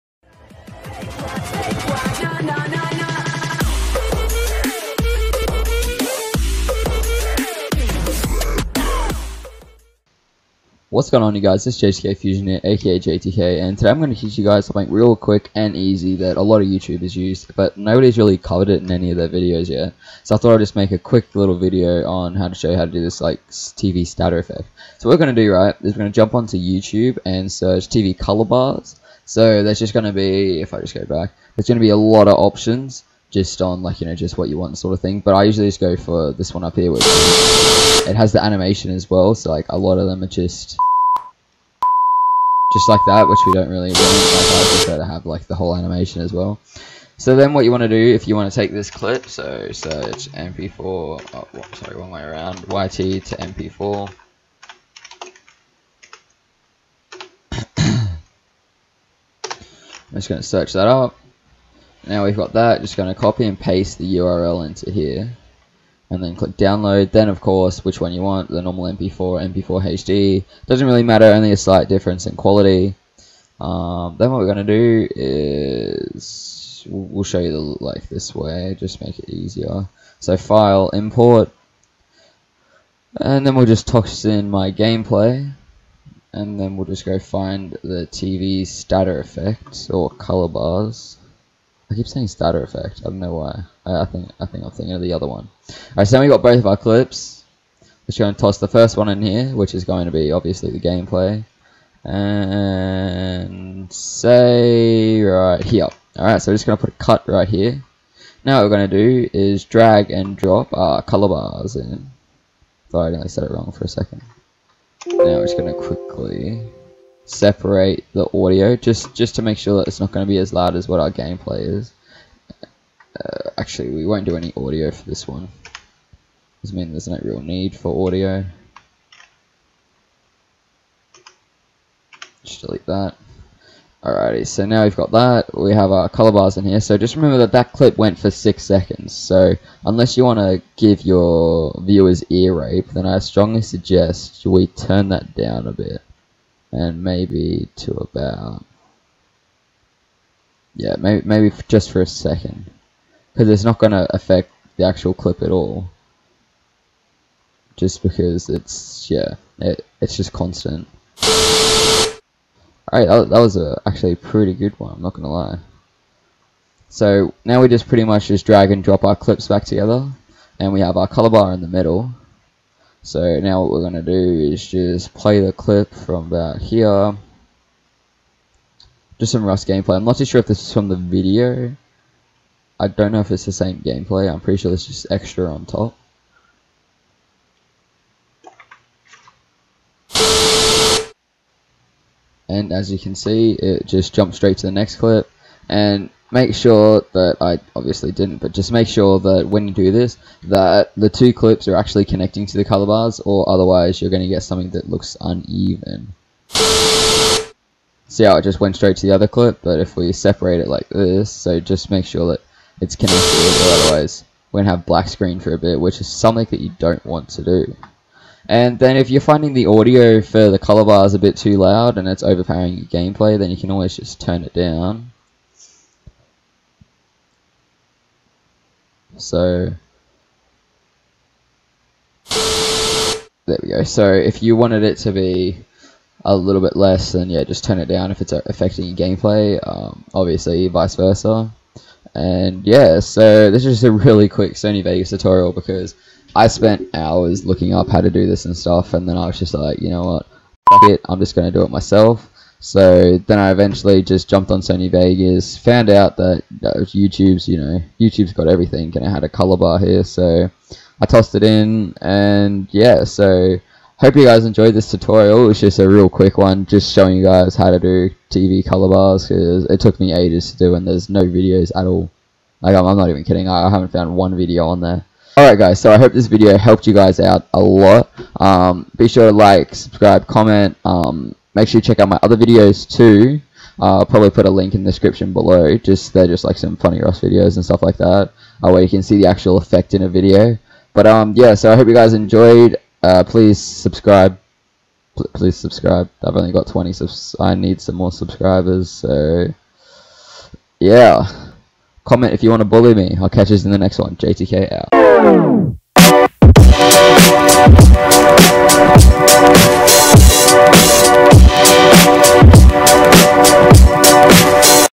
HOW TO MAKE A TV STATIC/COLOUR BAR EFFECT-((SONY VEGAS 13))
Hey guys this is just a quick video/tutorial on how to make the TV static effect/color bars effect. It is a really cool effect that i use and many other Youtubers used in compilation. This is my first tutorial so tell me how it is (Balake, Twitchy, Roasted). Enjoy the video.